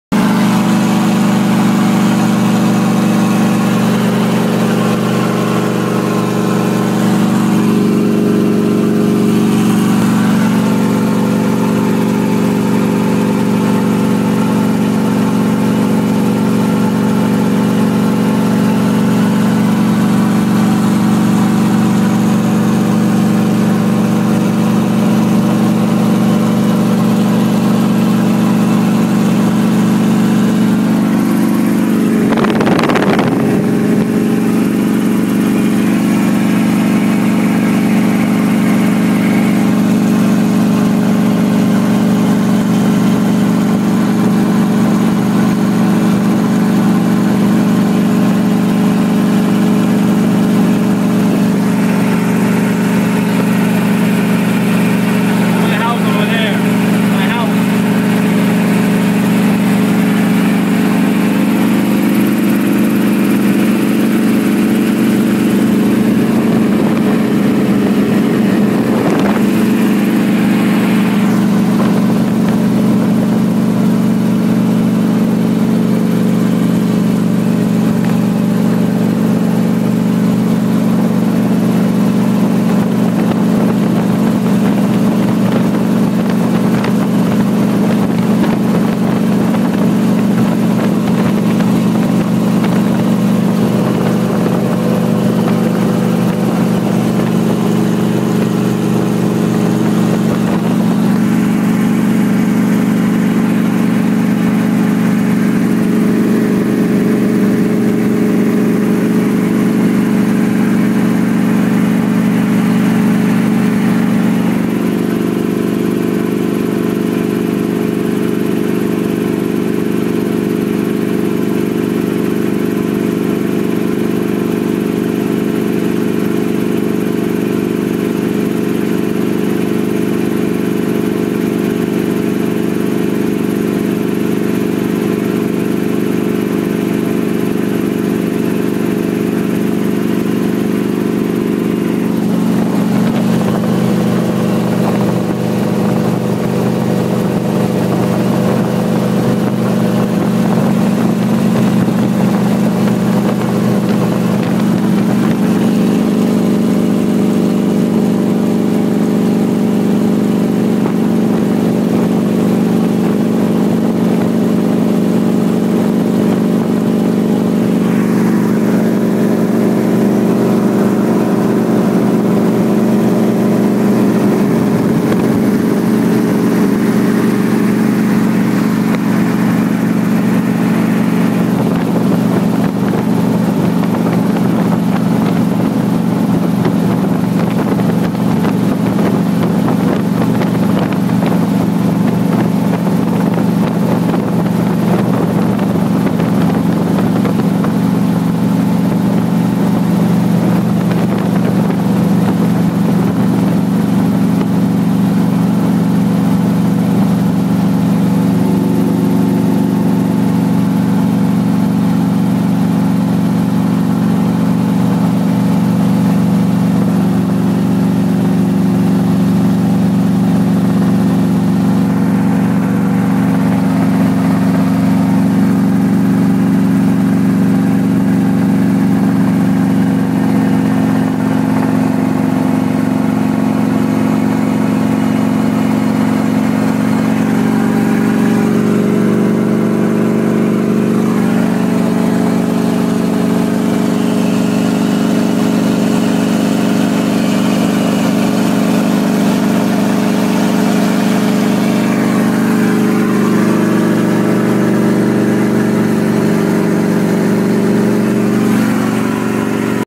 Helicopter ride
Raul first helicopter ride ever!!! It was awesome!!! I can do this all day!!